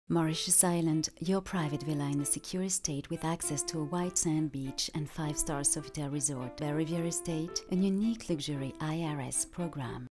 Stone Investment - Spot TV de Guillaume Didier et Richard Martineau
Spot TV diffusé sur Fashion TV depuis 09.2008 - Réalisé par Guillaume Didier - Production éxécutive: Richard Martineau - Agence: MCMC Production Ltd